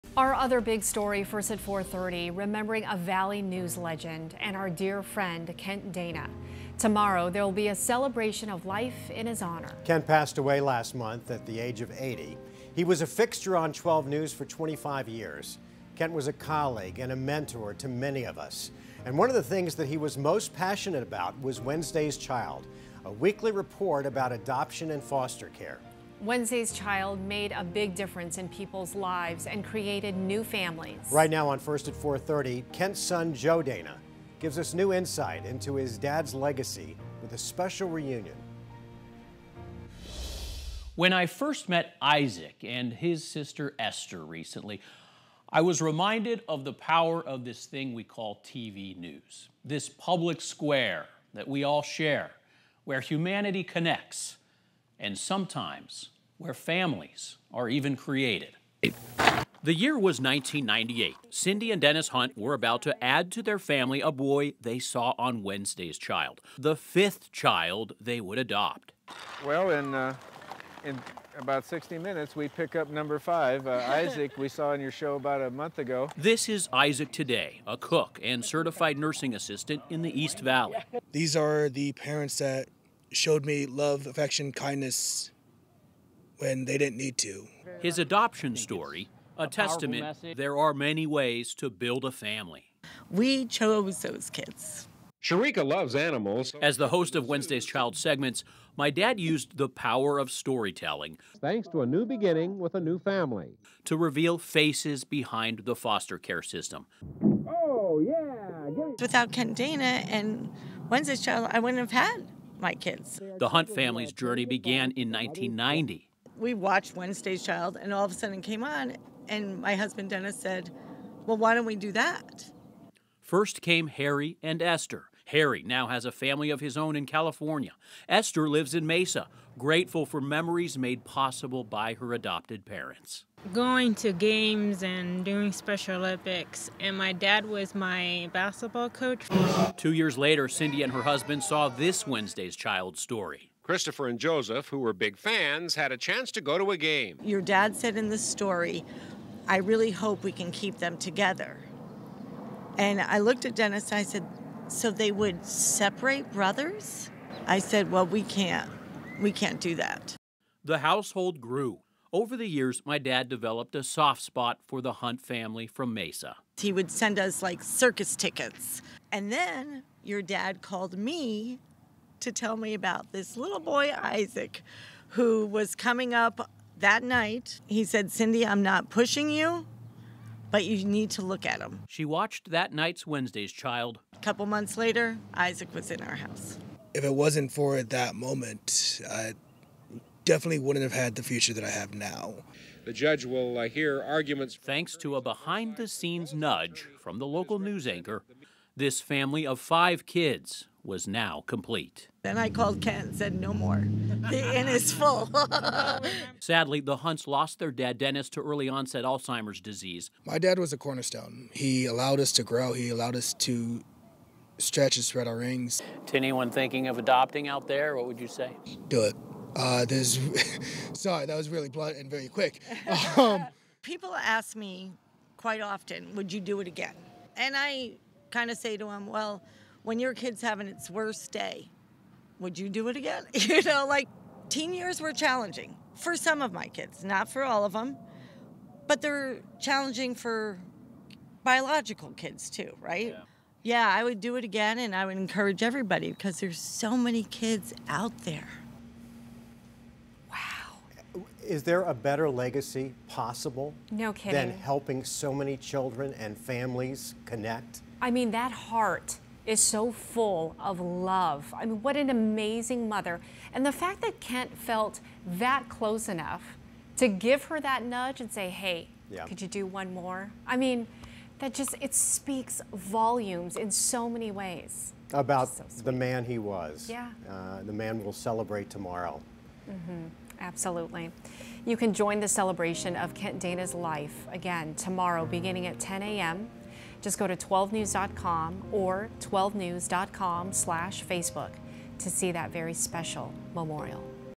Mesa family who adopted 5 children honors 12 News legend Kent Dana
As family and friends of the late Kent Dana hold a celebration of life in his honor, a Mesa family is remembering the TV anchor for the unique role he played in their lives.

“Without Kent Dana and ‘Wednesdays Child’ I wouldn’t have my kids,” said Cindy Hunt, a mother of five.

Dana passed away last month at the age of 80.